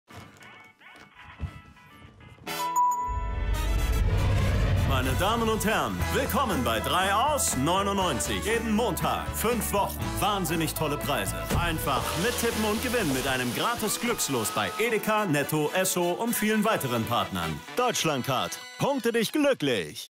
3 aus 99 - die neue DeutschlandCard-Kampagne
Der neue TV-Werbespot von DeutschlandCard sowie digitale Gamification-Aktionen begleiten die Kampagne. Das Mediabudget liegt im siebenstelligen Bereich. Für die Konzeption und Umsetzung zeichnet erneut die Serviceplan Gruppe verantwortlich.